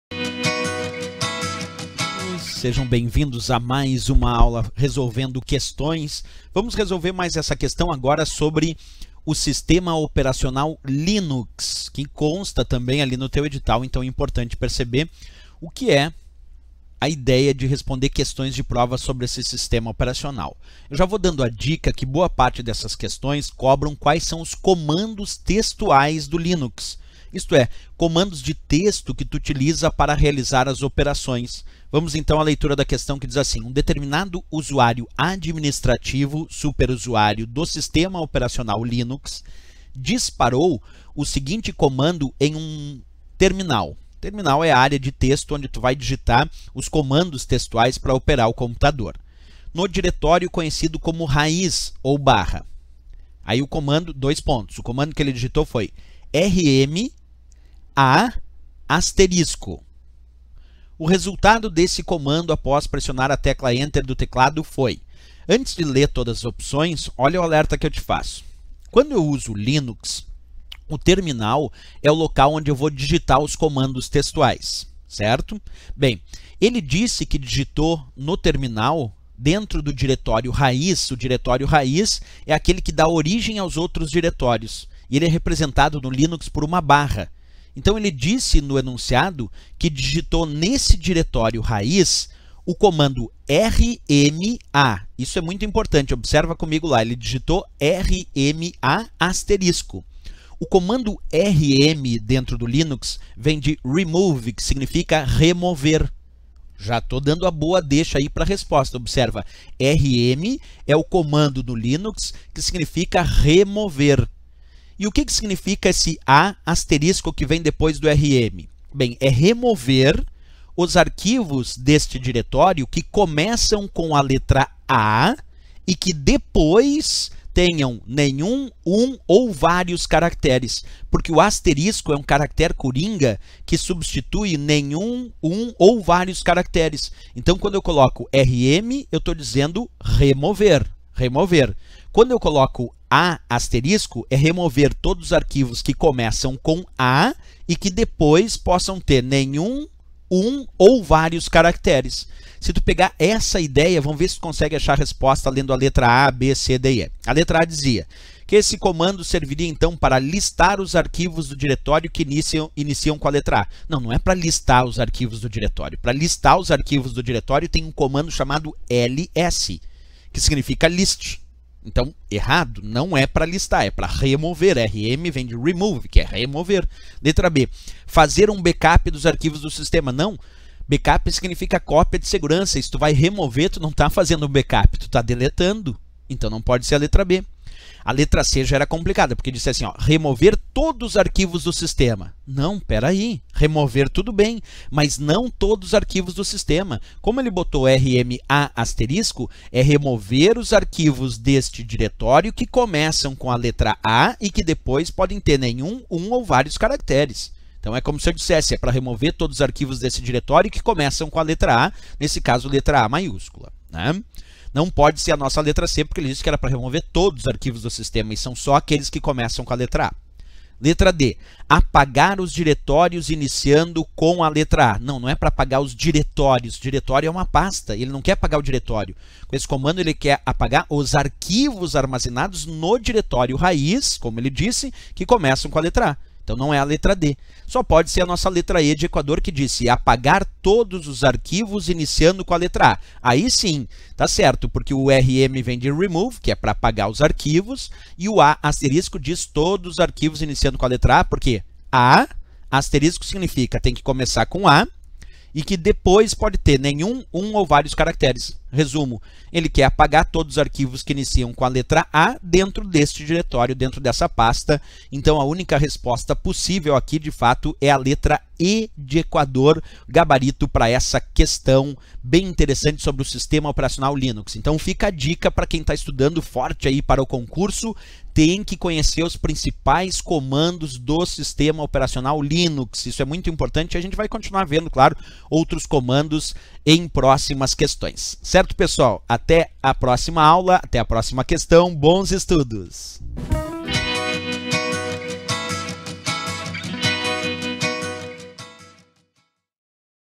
Linux para concursos e software livre - Aula 24 - Rodrigo Schaeffer
Conheça as características do software livre Linux e seus comandos para concursos.
Linux e uma questão sobre comandos de texto RM.
Apresentação: Rodrigo Schaeffer